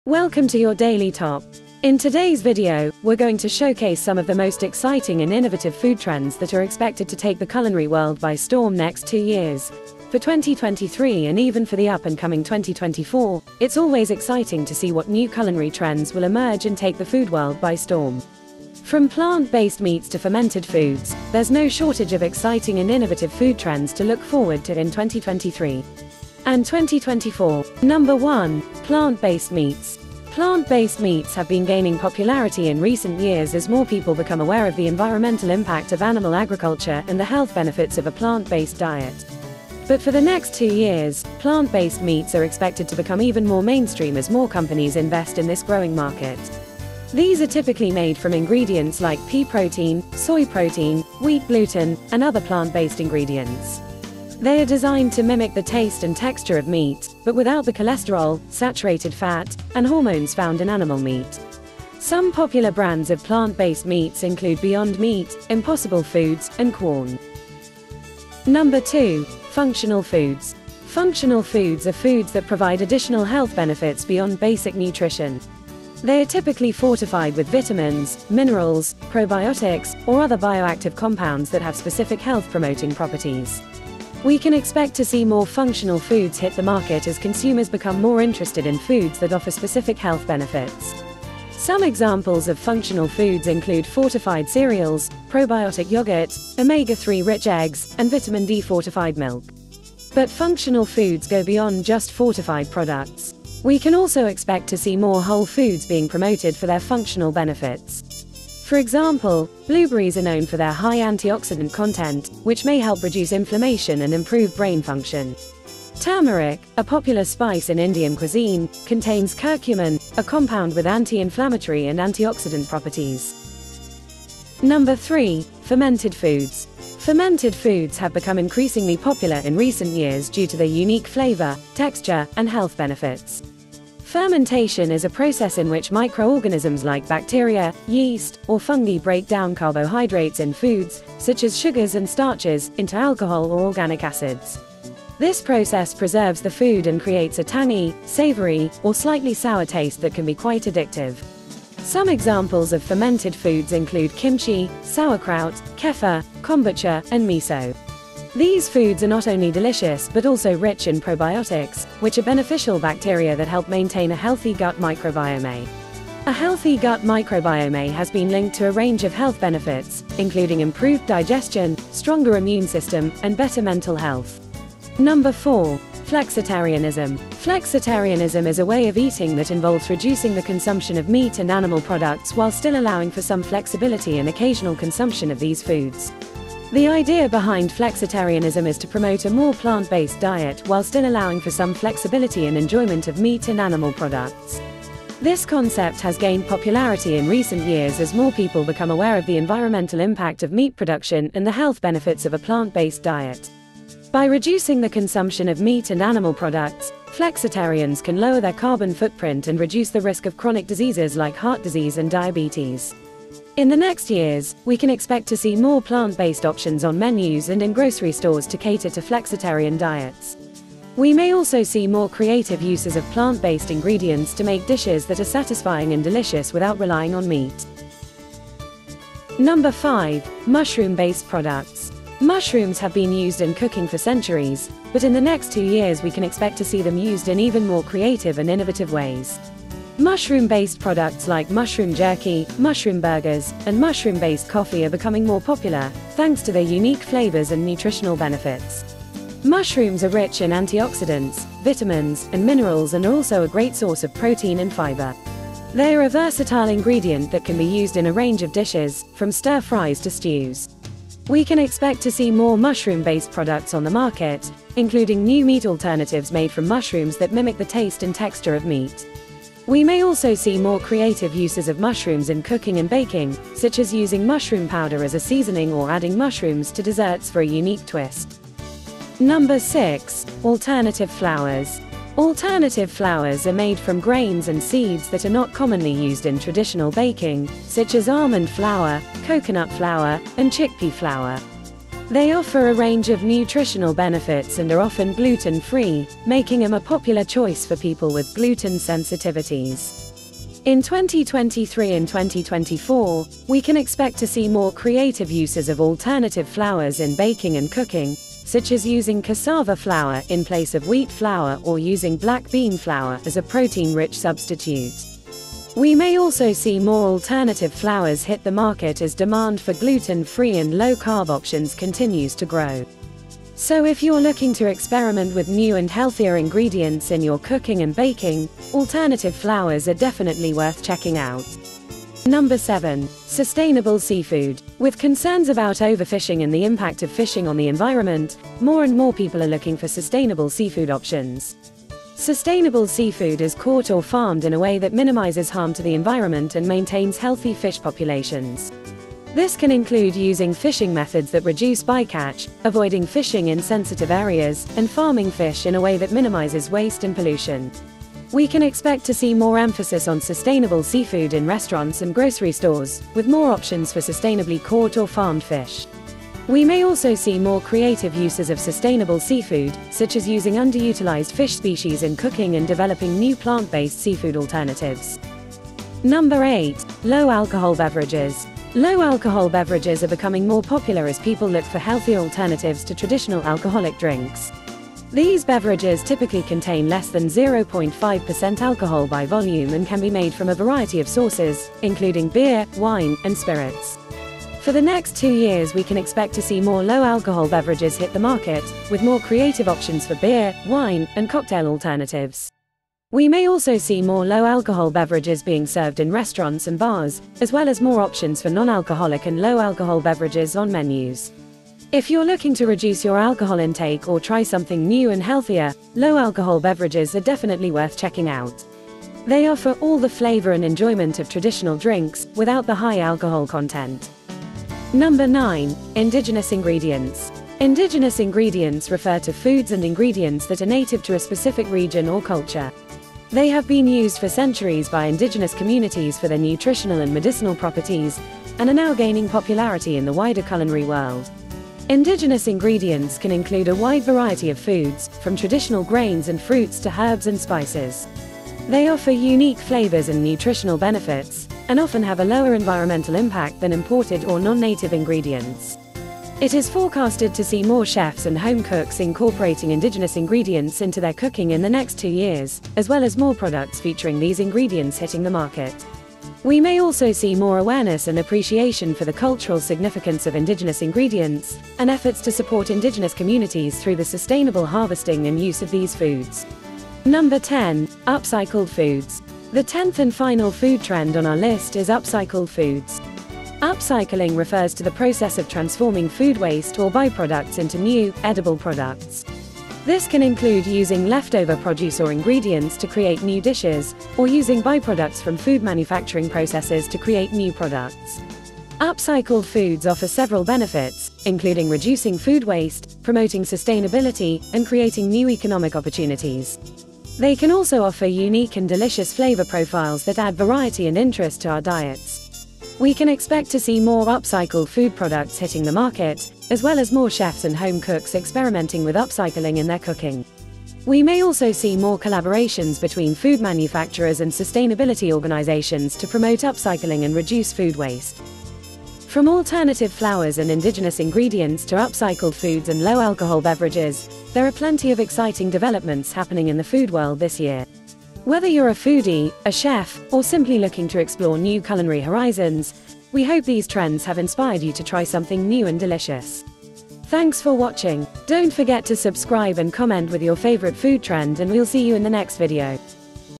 Healthy Foods | Top 10 Popular Food Trends You Need to Try in 2023 and 2024
Ever wondered what you should eat or what is actually a ''trend'' now in 2023?

We got you covered here !

In this video, we explore the top 10 popular food trends to watch in 2023 and 2024. From functional foods and sustainable seafood to fermented foods and upcycled foods, we take a deep dive into the culinary world and discover the exciting developments happening in food and drink. Join us as we explore the latest trends new food trends 2023, ingredients, and products that are shaping the way we eat and drink in the new year. These are popular food trends 2023! Take a look !